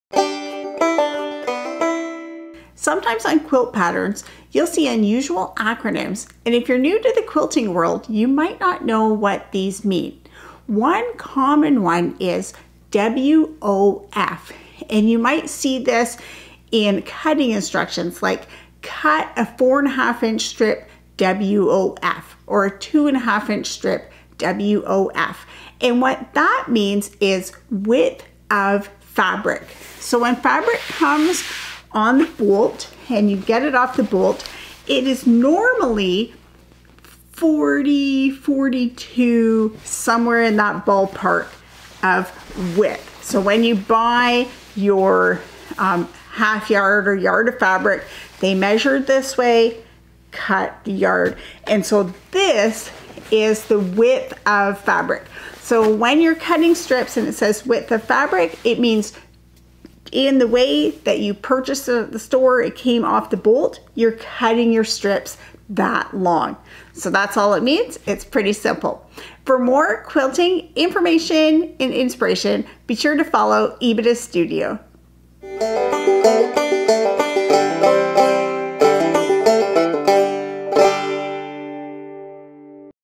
What does "WOF" on a quilting pattern mean?
Sometimes on a quilt pattern, you will see the term "WOF".
The meaning is actually pretty straightforward.